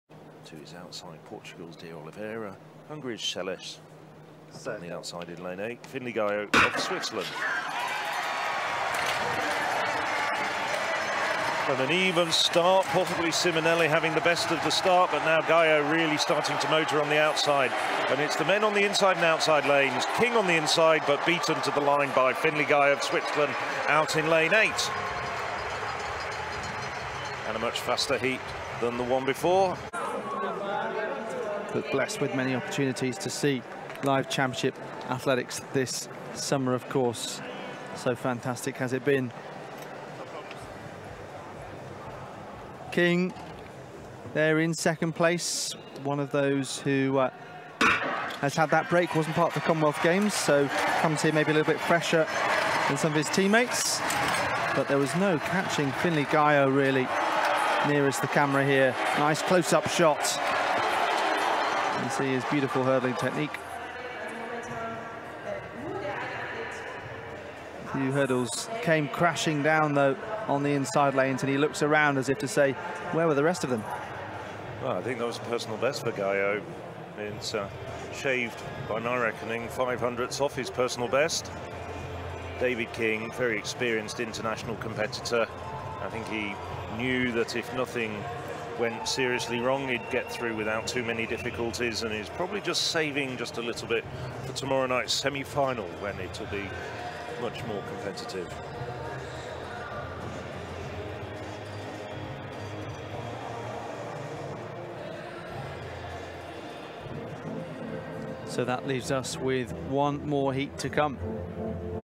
Dive in 110m hurdles - Joao Vitor De Oliveira qualifies by 0.002 second
What a dive! Munich - 110 metre hurdles - European championships
Diving -
Tauchen leichtathletik
Quel plongeon !
Que mergulho!
Que inmersión!
Was für ein Sprung!
好一个潜水!
なんという飛び込み
Athletics - Athlé - Athlétisme